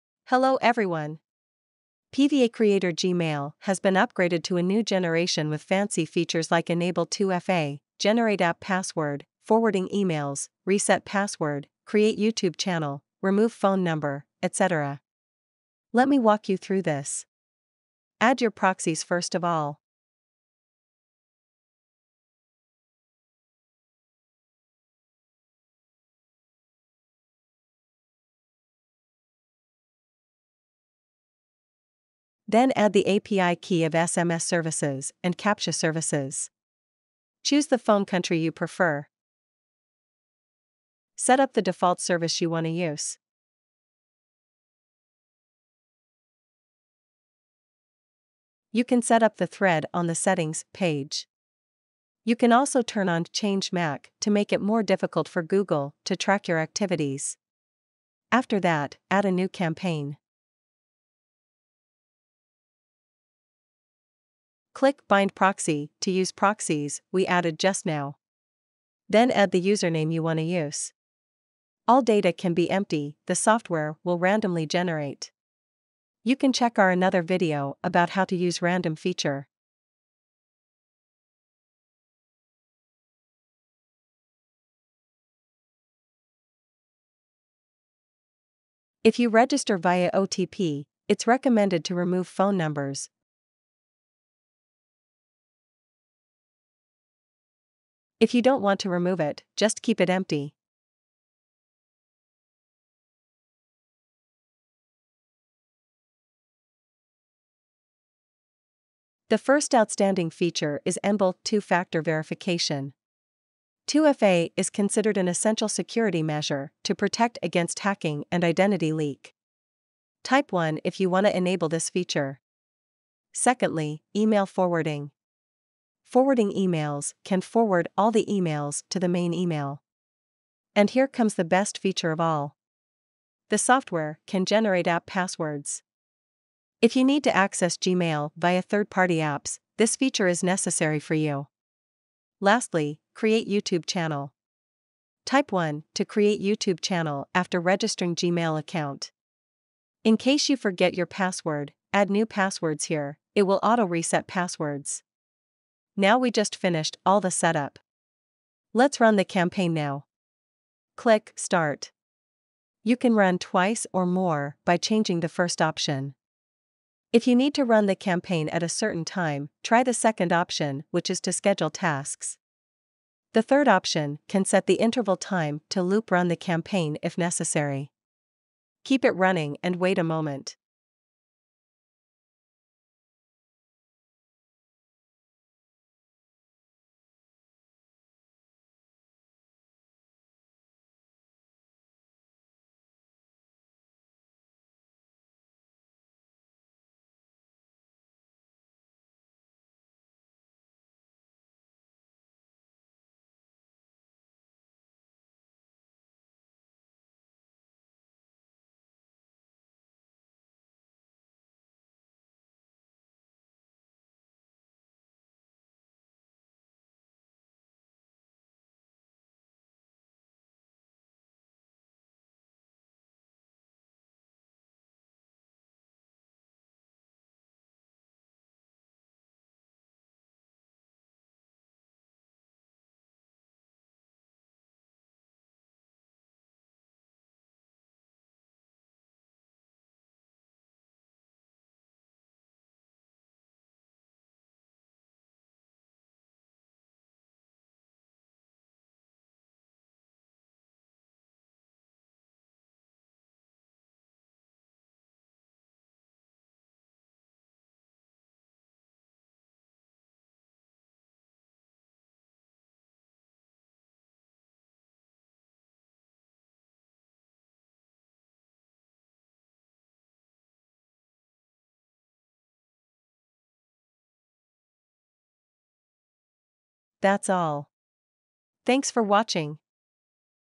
Gmail Account Creator | Gmail Registration Tool | How to Create Unlimited Gmail Accounts Easily
is a software which allows you create unlimited Gmail email accounts in seconds. It will automate the process of creating bulk Gmail accounts.
Main features:
Create bulk gmail accounts
Auto farm created gmail accounts (Create YouTube channel, Create Google files, Update Avatar, Send Email...)
Support using Dcom or Proxies to change IP
Export gmail result file in many formats: Txt, csv
Built-in Scheduler tool to schedule process
Create Phone Verified Gmail Accounts
Auto Rent phone number from SMS Providers
Multi-threads